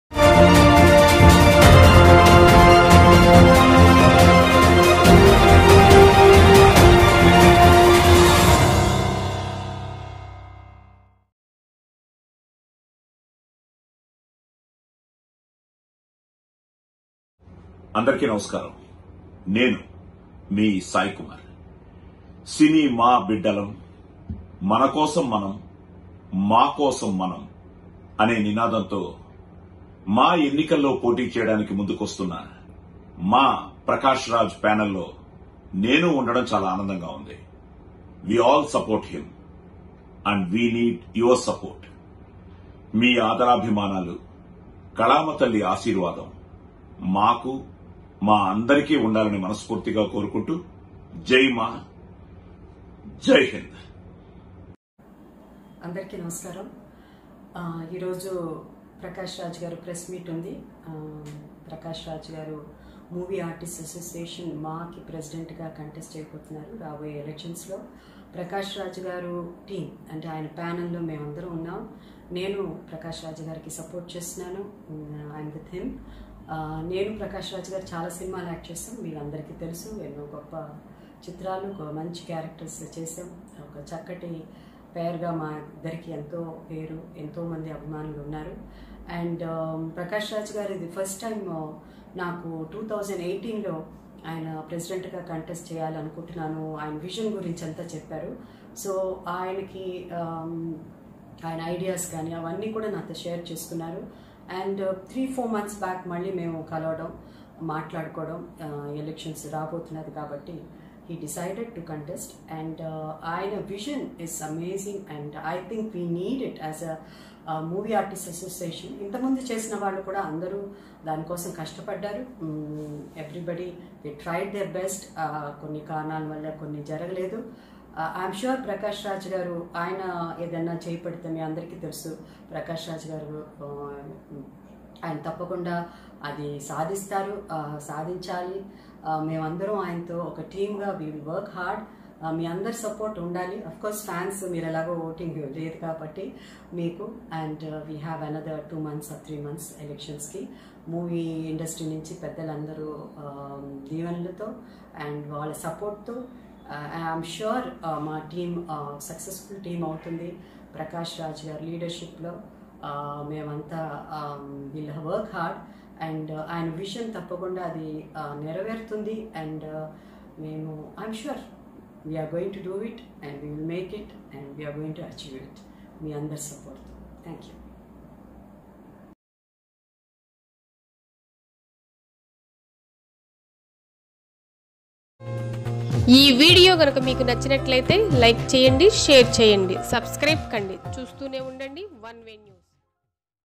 మా ఎన్నికలపై జయసుధ, సాయి కుమార్ లు ఏమన్నారంటే? ప్రకాష్ రాజ్ ఒక కొత్త విజన్ తో ముందుకు Maa Elections
మా ఎన్నికల్లో మా టీమ్ దే విజయమంటున్న జయసుధ, సాయి కుమార్ లు.. ప్రకాష్ రాజ్ ఒక కొత్త విజన్ తో ముందుకు వస్తున్నారు. Maa Elections